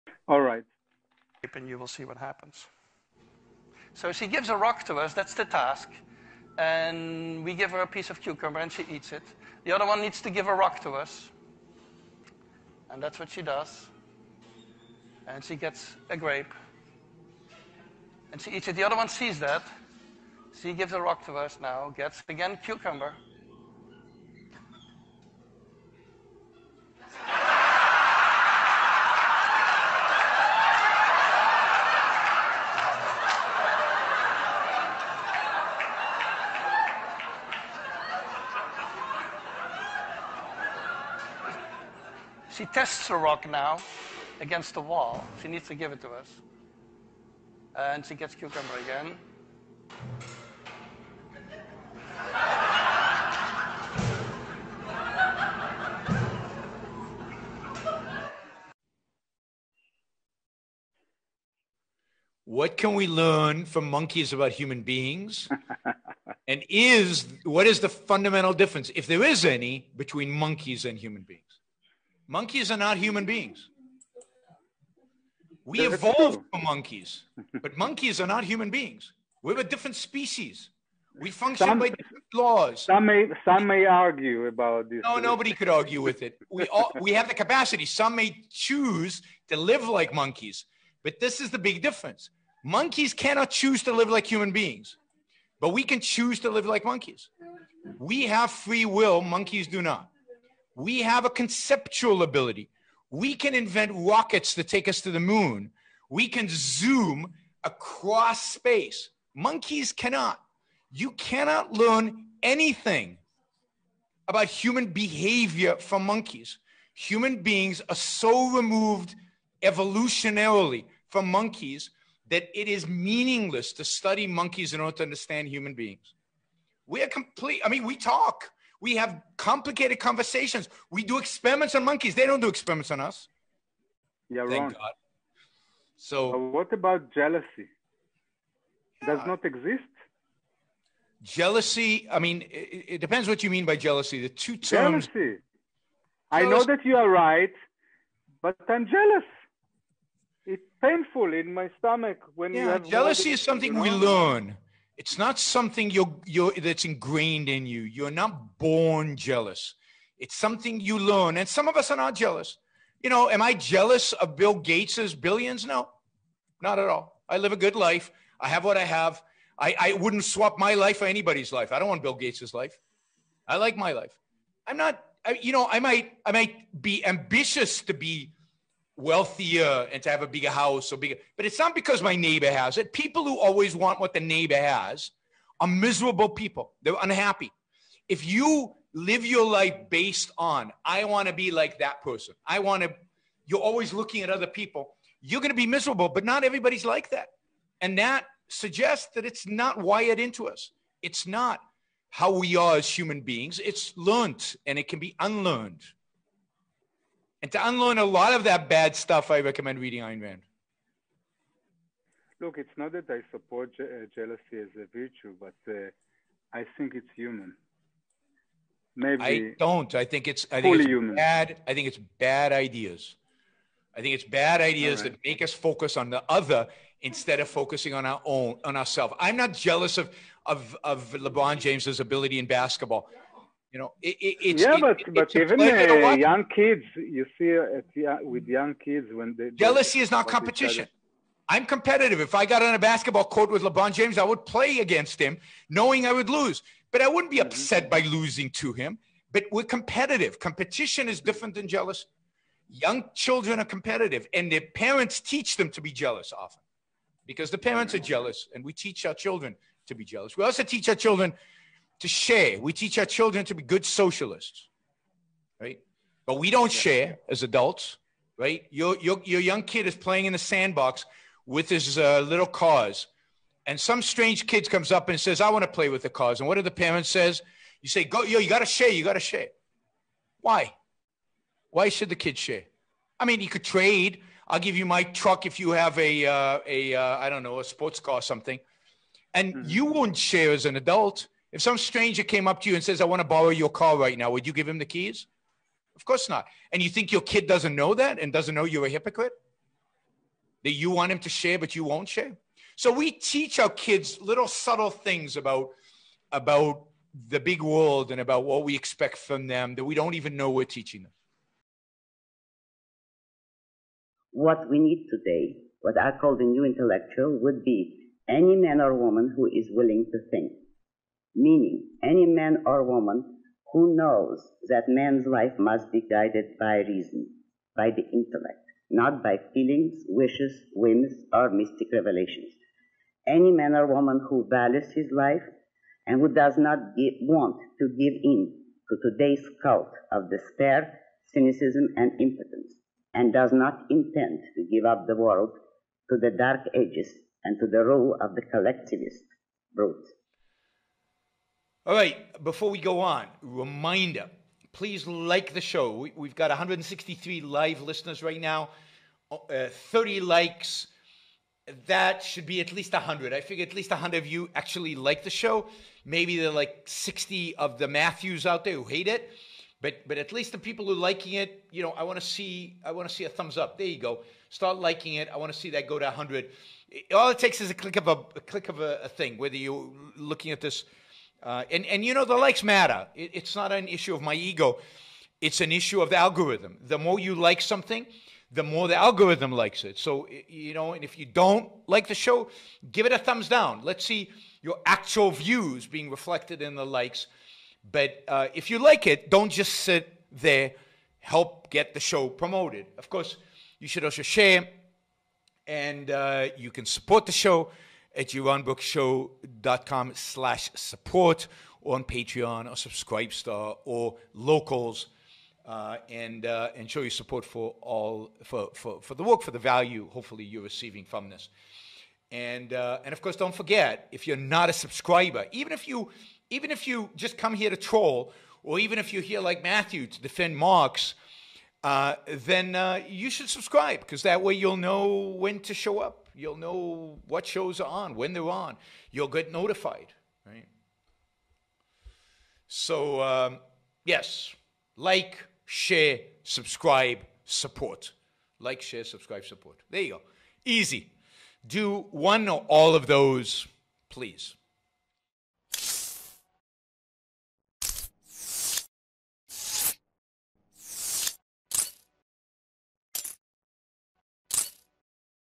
What Can We Learn from the Capuchin Monkey Experiment?
Become a monthly contributor This video was created by Christian Jackson.  Taken from Yaron Lectures: Is Economic Inequality a Problem? Recorded on June 29, 2020.